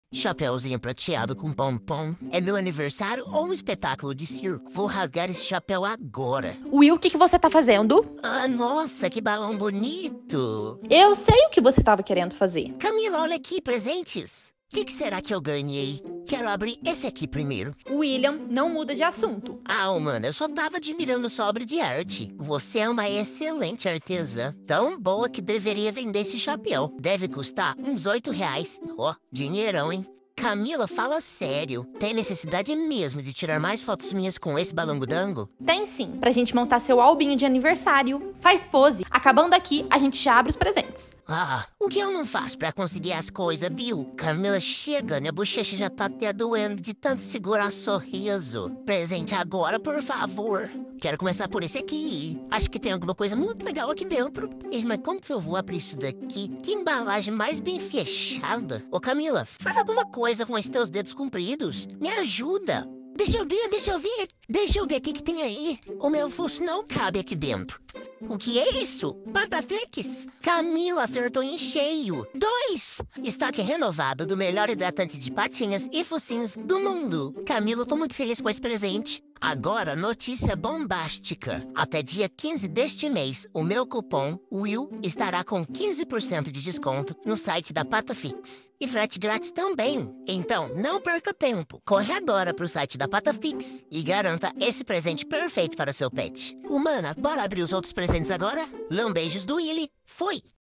Meu cachorro abrindo os presentes que ele ganhou de aniversário | PataFix
Oiii gente, como vocês estão?
No vídeo de hoje vou abrir meus presentes de aniversário com vocês
Na verdade um dos presentes, volto em breve pra mostrar os outros
Aproveitem! Não percam esse descontaço!
Espero que se divirtam assistindo
Aubraços & lambeijos do Willy ❤️